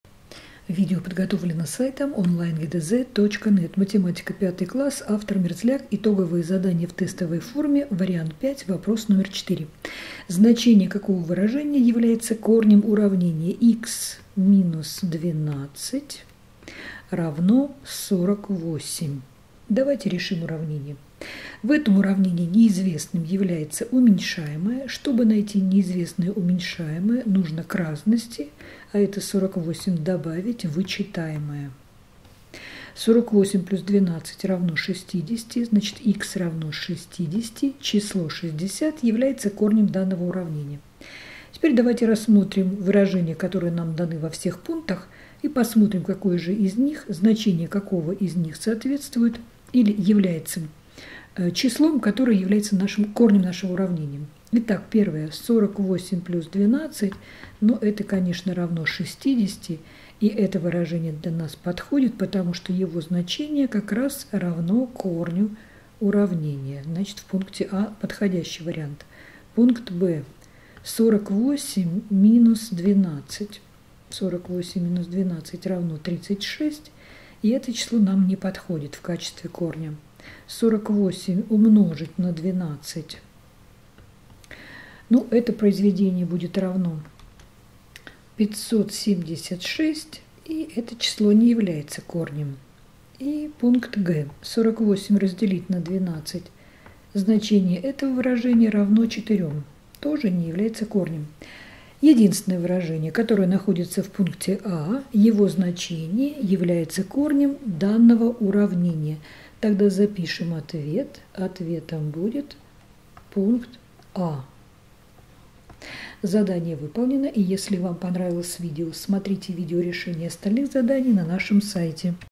Задание №4 Вариант 5 Итоговые задания - Математика 5 класс (Мерзляк А.Г., Полонский В.Б., Якир М.С.)
Так как в уравнении, данном в задании 4, буква х стоит на месте уменьшаемого, то для того, чтобы найти корень уравнения, нужно применить правило нахождения неизвестного уменьшаемого. Рекомендуем воспользоваться нашим видео-решением для быстрого и эффективного решения задания.